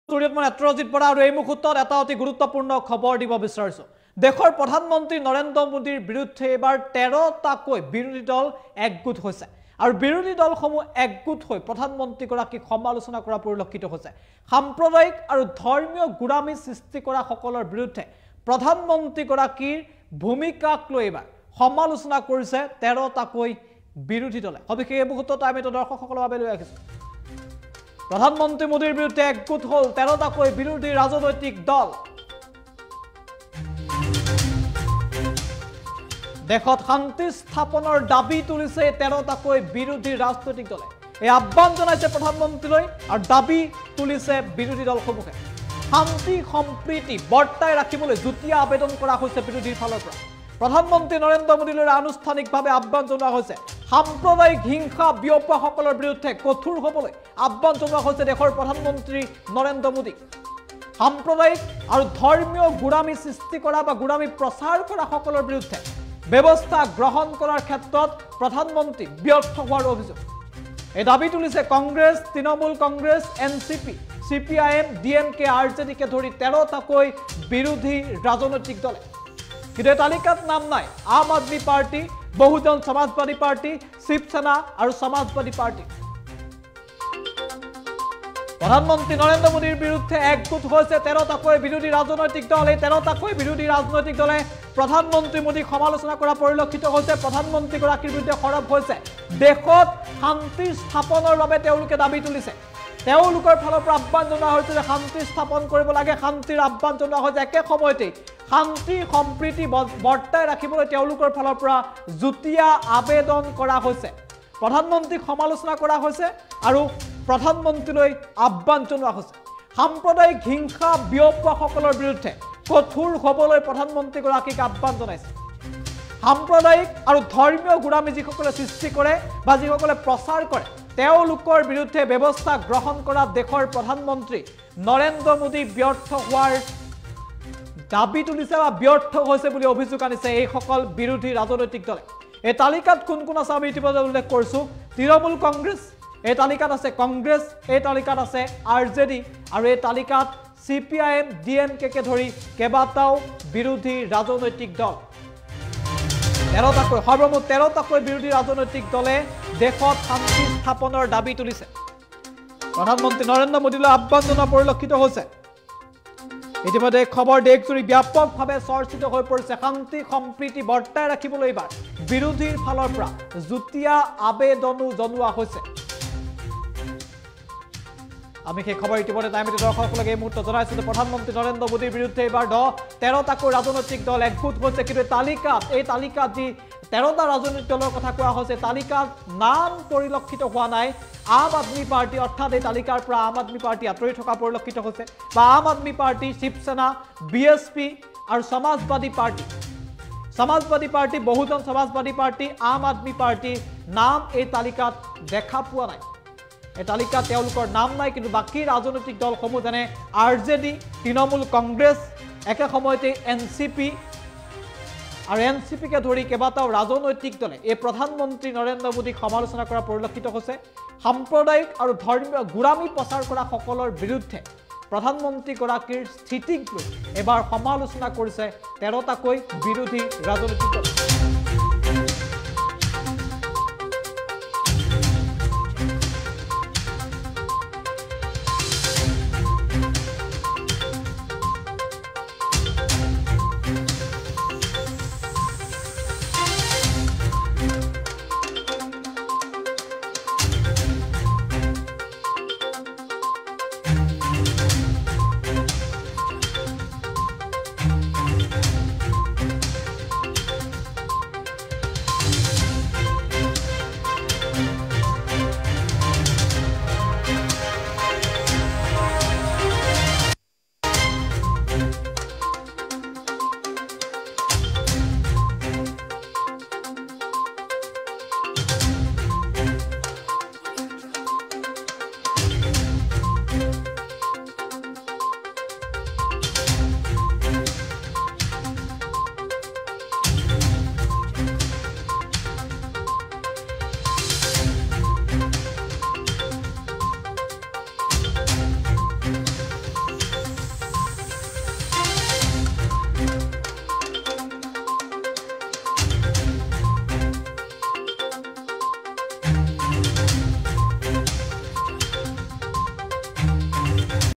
প্ৰধান মন্ত্ৰী মোদীৰ বিৰুদ্ধে একগোট ১৩ বিৰোধী দল
প্ৰধান মন্ত্ৰী মোদীৰ বিৰুদ্ধে  একগোট ১৩ বিৰোধী দল। সাম্প্ৰদায়িক হিংসা বিয়পোৱাসকলৰ বিৰুদ্ধে কঠোৰ হ'বলৈ আহ্বান মোদীক। সাম্প্ৰদায়িক, ধৰ্মীয় গোড়ামি প্ৰচাৰ কৰাসকলৰ বিৰুদ্ধে ব্যৱস্থা গ্ৰহণত প্ৰধান মন্ত্ৰী ব্যৰ্থ হোৱাৰ অভিযোগ